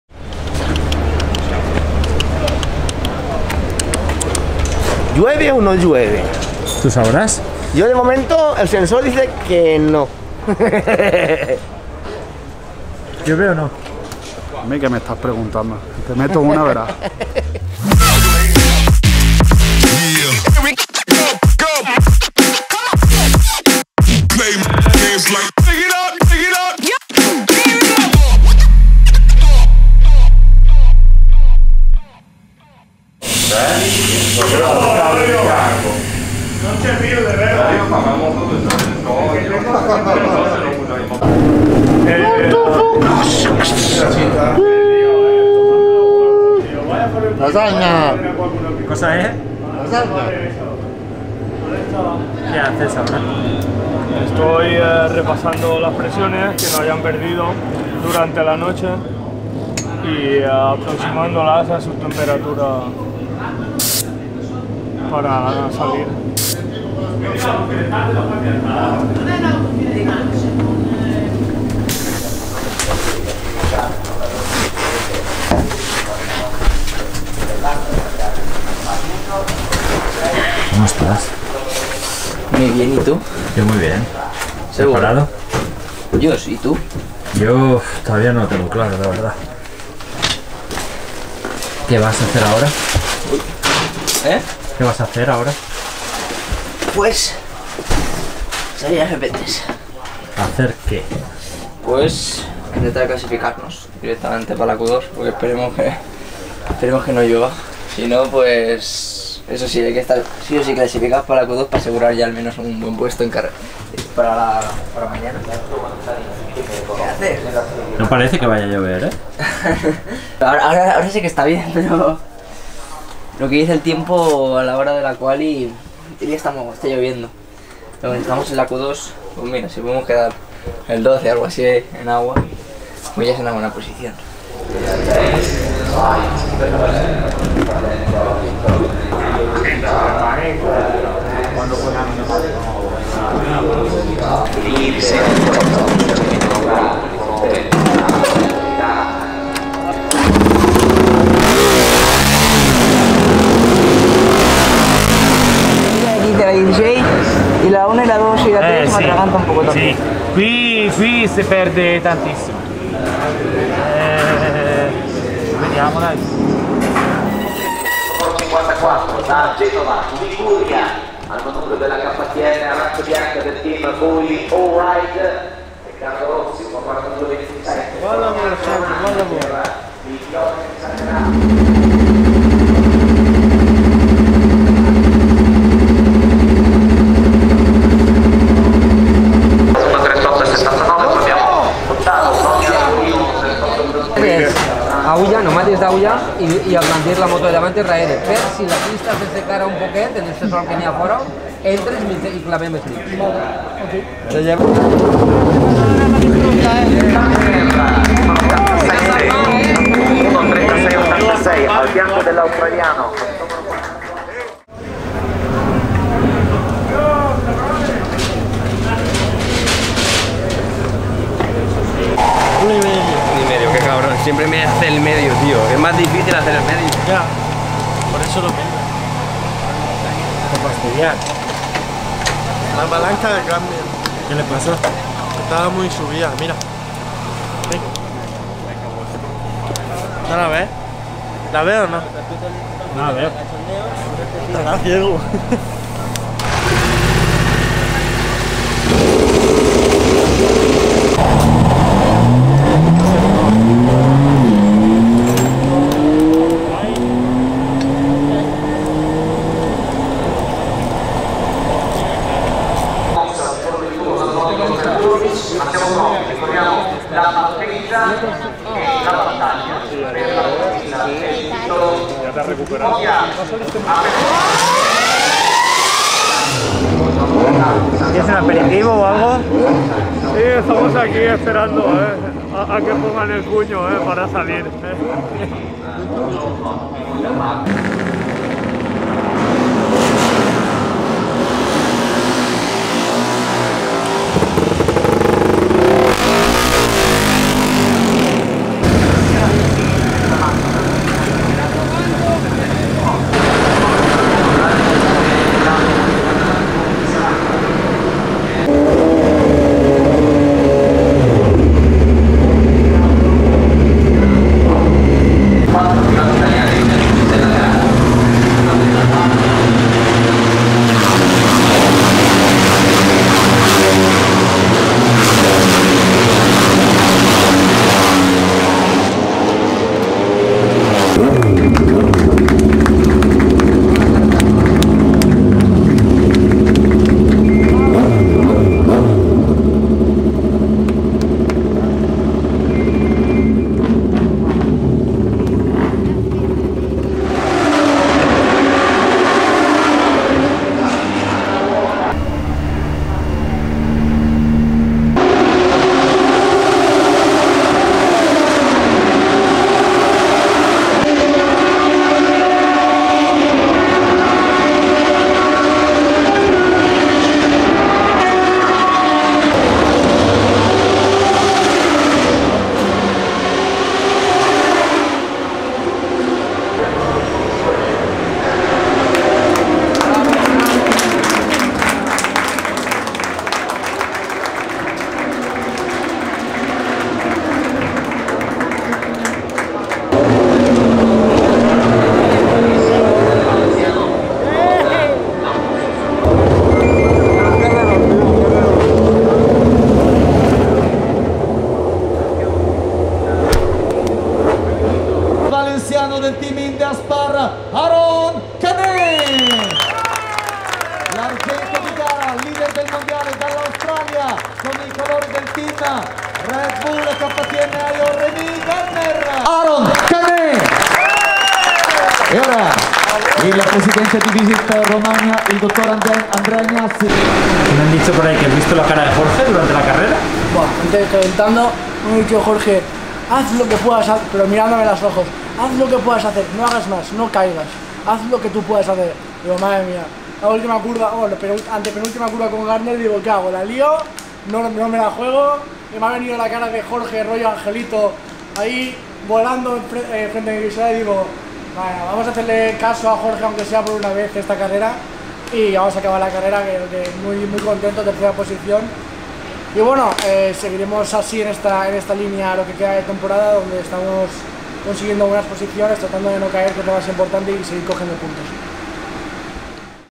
Inside Aspar 15 - Podium at the SanMarinoGP
Vive desde dentro el gran premio de San Marino con Aspar Team y disfruta del podio de Aron Canet en el circuito de Misano Marco Simoncelli. Descubre qué visión tuvo Aron en las últimas curvas del circuito y conoce nuestro sensor de lluvia secreto en el episodio 15 de InsideAspar.

Experience the San Marino Grand Prix from within with Aspar Team and enjoy Aron Canet's podium at the Misano Marco Simoncelli circuit. Find out what vision Aron had in the last corners of the circuit and get to know our secret rain sensor in episode 15 of InsideAspar.